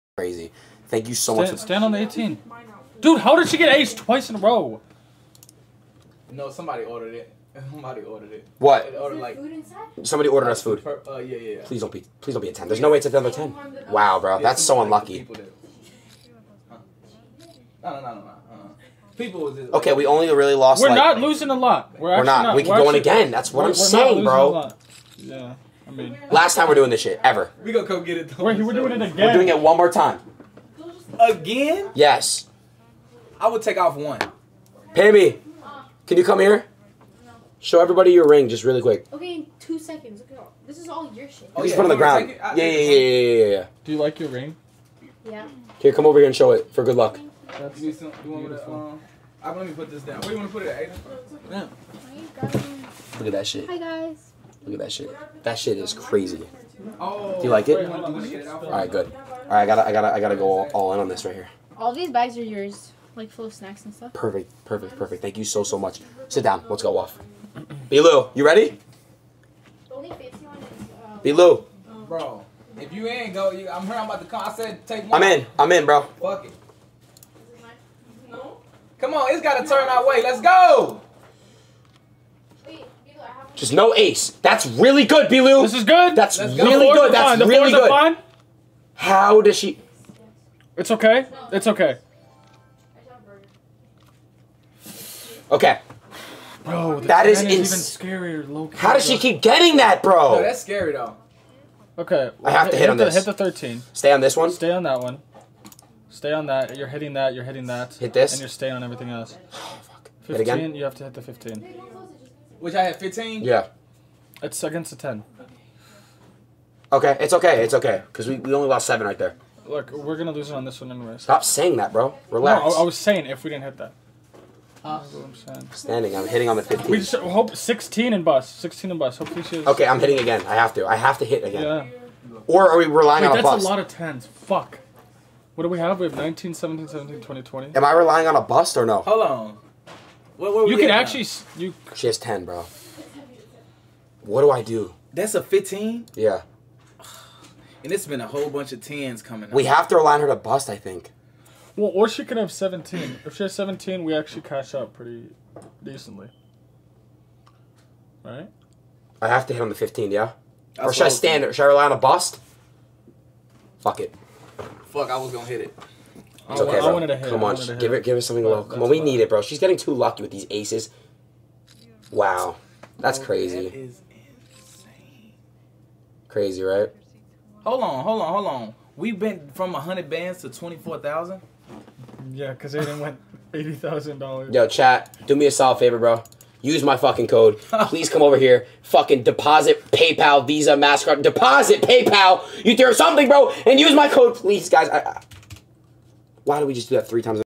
PamiBaby Pulls Up On Adin’s Stream & Shows Her Ring For Good Luck & This Happens
Follow me on Twitter @YT-TheByePlug *Copyright Disclaimer Under Section 107 of the Copyright Act 1976, allowance is made for "fair use" for purposes such as criticism, comment, news reporting, teaching, scholarship, and research.